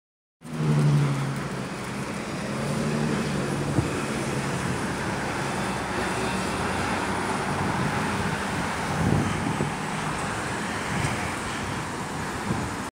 [MTA]:Kings Plaza Bound 2018 New Flyer XD40 'Xcelsior' [7630] On The B46 +SBS+
[MTA New York City Transit]:Flatbush Depot

B46 +SBS+:Kings Plaza To/From Dekalb Avenue

(Fleets):2018 New Flyer XD40 'Xcelsior' #7630